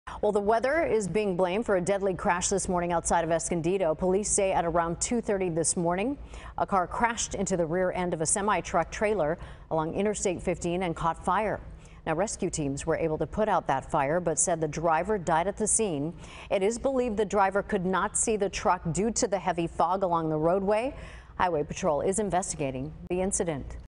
Fog Believed To Be Factor In Deadly Crash
Heavy fog is a suspected factor in the death of a man who crashed his car into a trailer attached to a big rig early Thursday morning.